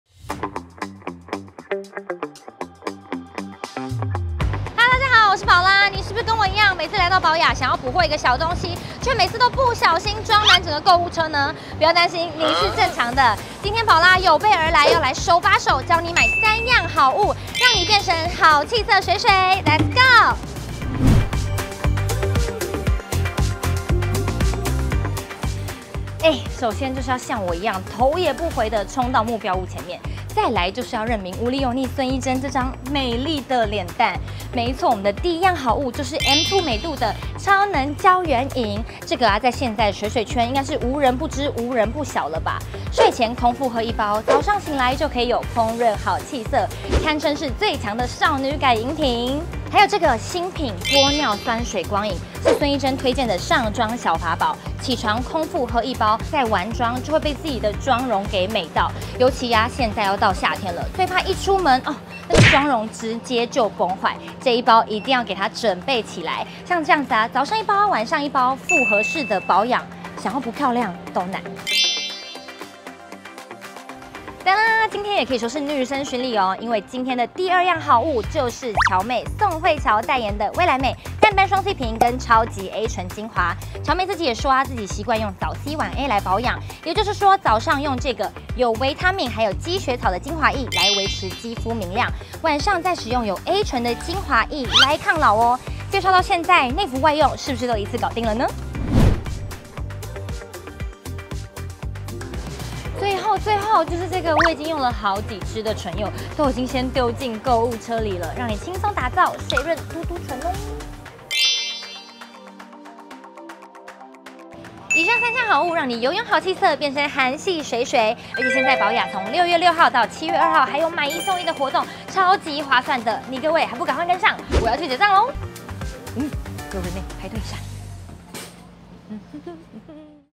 盤點寶雅3好物秒變身「韓系水水」！ 6/6~7/2買一送一！
不知道大家是不是每次來到寶雅，原本只是想要補貨一個小東西，結果卻不小心裝滿整個購物車呢！這次寶拉已經做好功課！手把手教你買三樣好物就能變成好氣色水水！